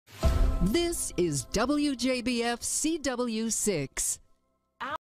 WJBF-DT3 Station ID (2024)
FINALLY A BRAND NEW CW THAT ISN'T LAZY WITH THE ID
This is WJBF-DT3's station ID from 2024
WJBF is located in Augusta, GA